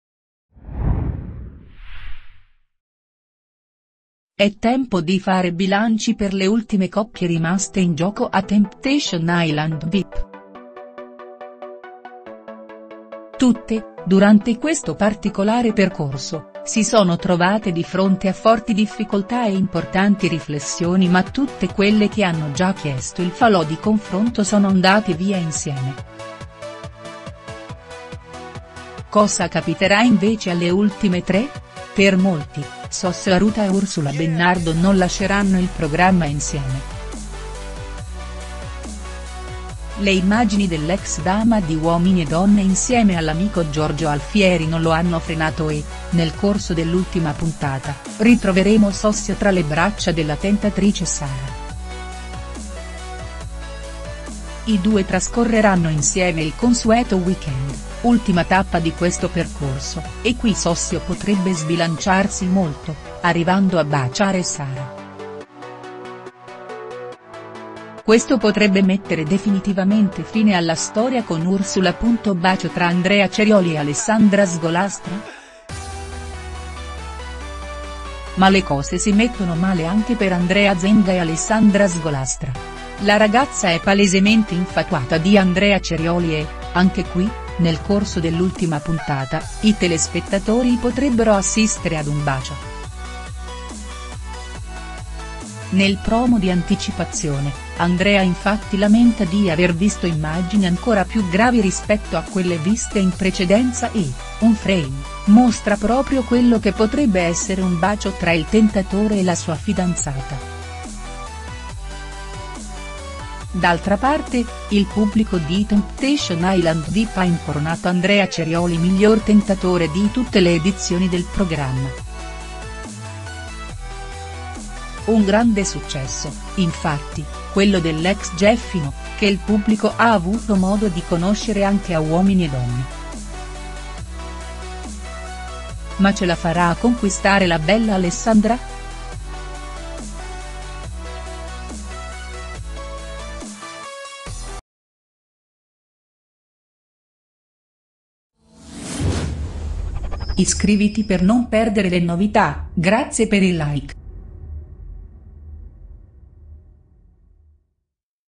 Temptation Island Vip/Sossio Aruta e Alessandra Sgolastra: scatta il bacio col tentatore? Gli indizi
È tempo di fare bilanci per le ultime coppie rimaste "in gioco" a Temptation Island Vip. Tutte, durante questo particolare percorso, si sono trovate di fronte a forti difficoltà e importanti riflessioni ma tutte quelle che hanno già chiesto il falò di confronto sono andati via insieme. Cosa capiterà invece alle ultime tre? Per molti, Sossio Aruta e Ursula Bennardo non lasceranno il programma insieme. Le immagini dell'ex dama di Uomini e Donne insieme all'amico Giorgio Alfieri non lo hanno frenato e, nel corso dell'ultima puntata, ritroveremo Sossio tra le braccia della tentatrice Sara. I due trascorreranno insieme il consueto weekend, ultima tappa di questo percorso, e qui Sossio potrebbe sbilanciarsi molto, arrivando a baciare Sara. Questo potrebbe mettere definitivamente fine alla storia con Ursula.

BACIO TRA ANDREA CERIOLI E ALESSANDRA SGOLASTRA?
Ma le cose si mettono male anche per Andrea Zenga e Alessandra Sgolastra. La ragazza è palesemente infatuata di Andrea Cerioli e, anche qui, nel corso dell'ultima puntata, i telespettatori potrebbero assistere ad un bacio. Nel promo di anticipazione, Andrea infatti lamenta di aver visto immagini ancora più gravi rispetto a quelle viste in precedenza e, un frame, mostra proprio quello che potrebbe essere un bacio tra il tentatore e la sua fidanzata. D'altra parte, il pubblico di Temptation Island Vip ha incoronato Andrea Cerioli "Miglior tentatore di tutte le edizioni del programma". Un grande successo, infatti, quello dell'ex gieffino, che il pubblico ha avuto modo di conoscere anche a Uomini e Donne. Ma ce la farà a conquistare la bella Alessandra?